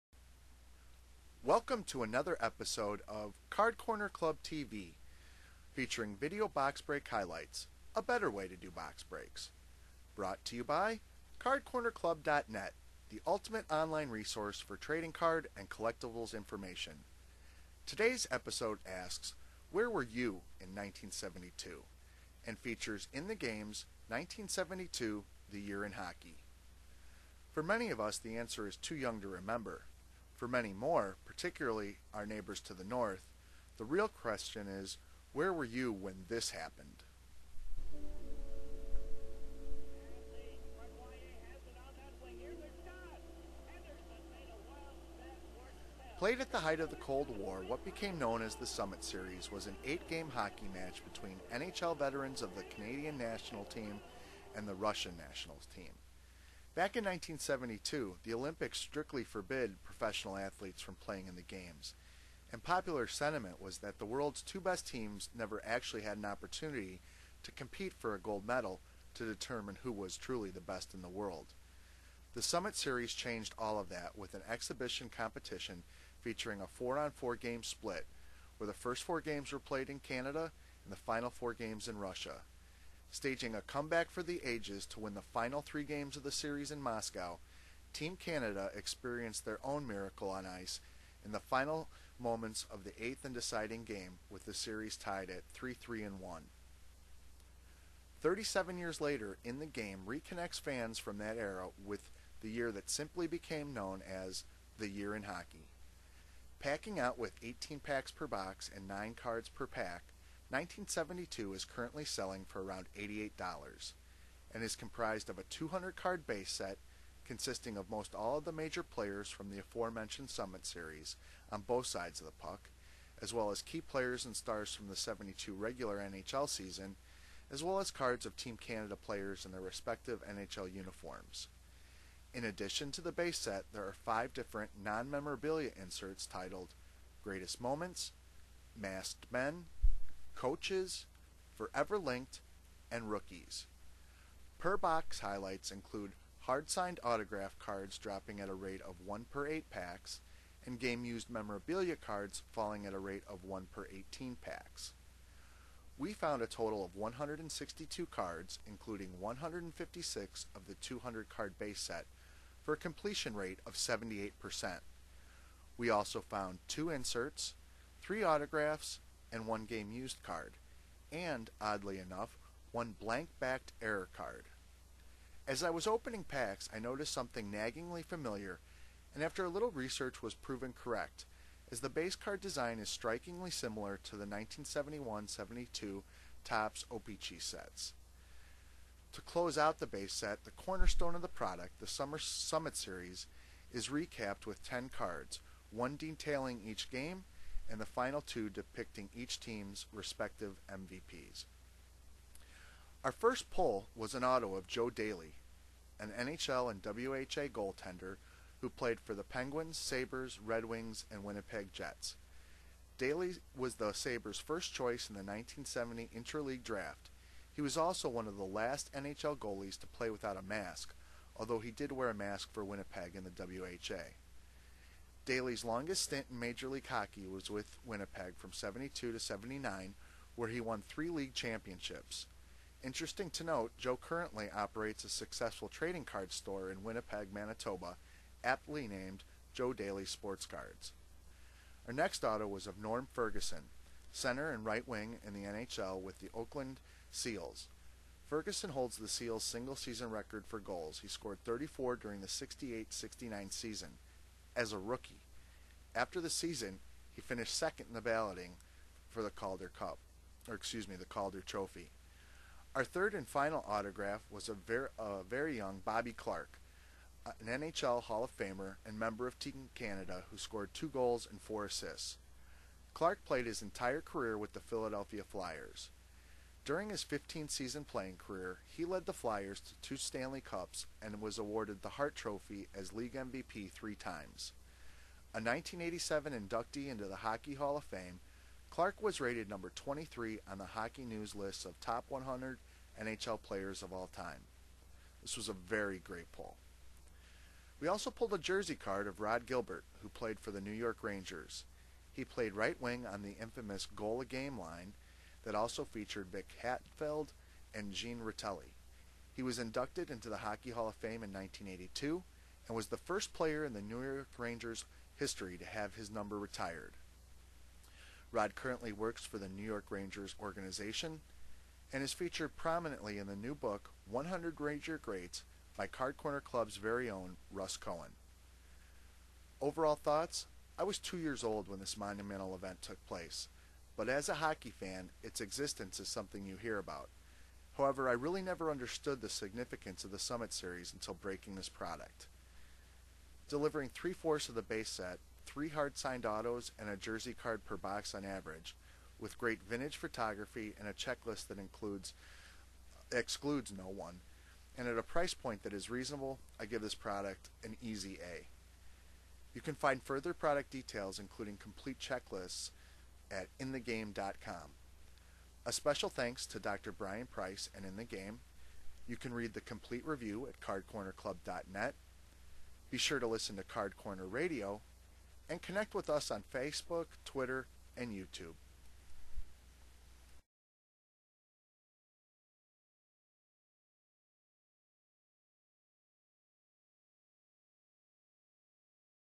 ITG's 1972 The Year In Hockey Video Box Break Highlights
In The Game brings fans back to 1972- The Year In Hockey. Video box break highlights from the recently released product from ITG